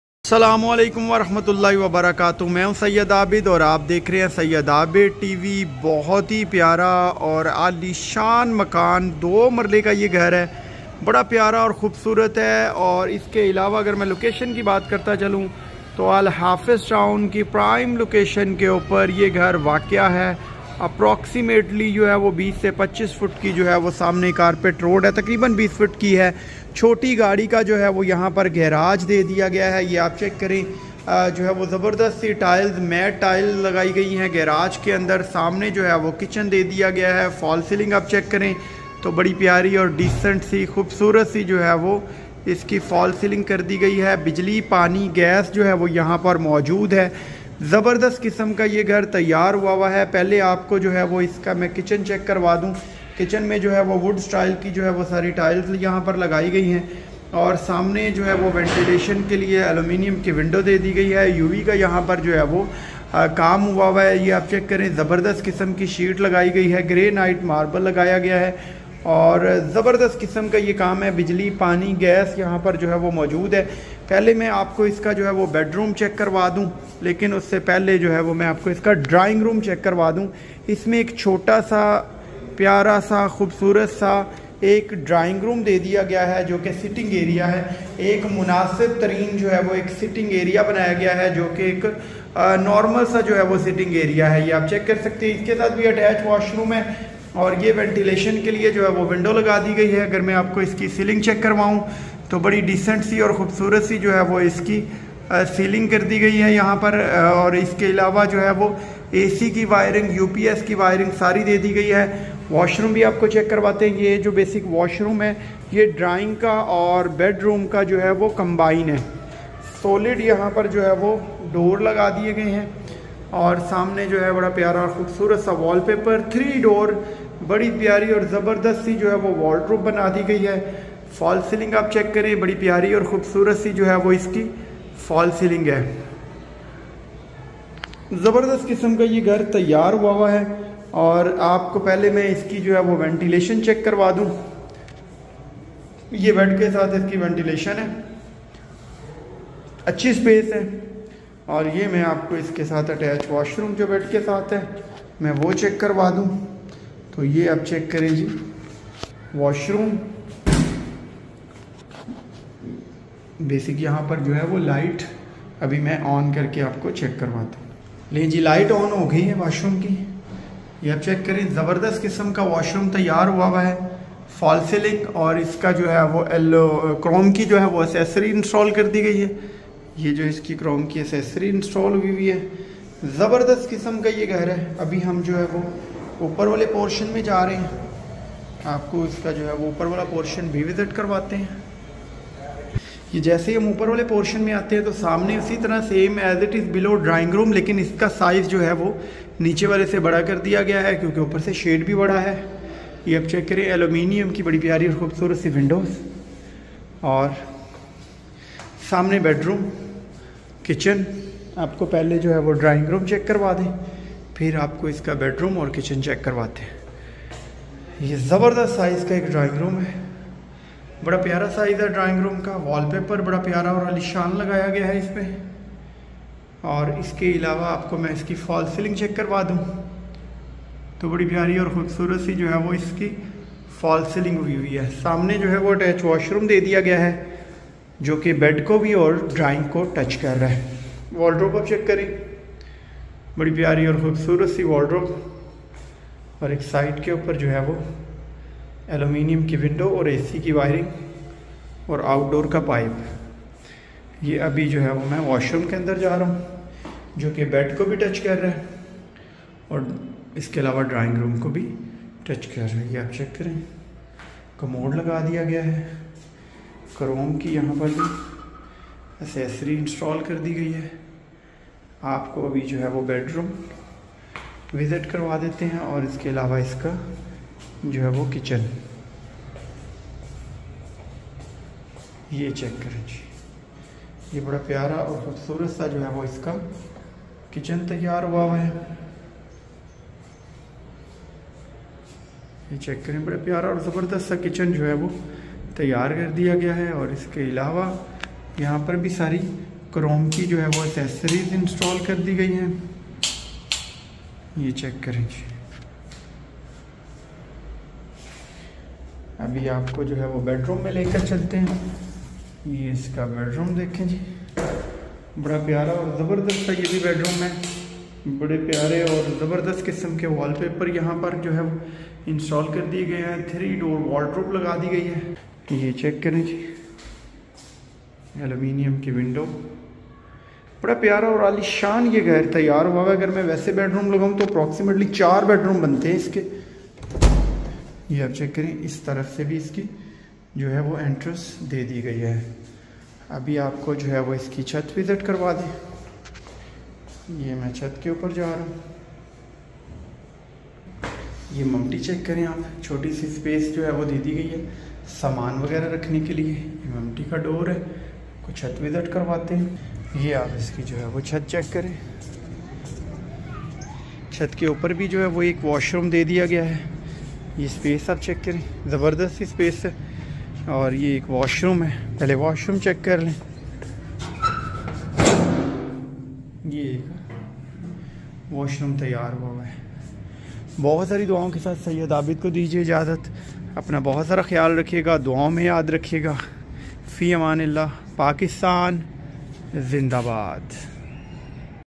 2 مرلہ برینڈنیو گھر 4 بیڈ رومز نیر مین 80 فٹ مرغزار روڈ
2 marla house near marghzar road
demand 75 lac
negotiable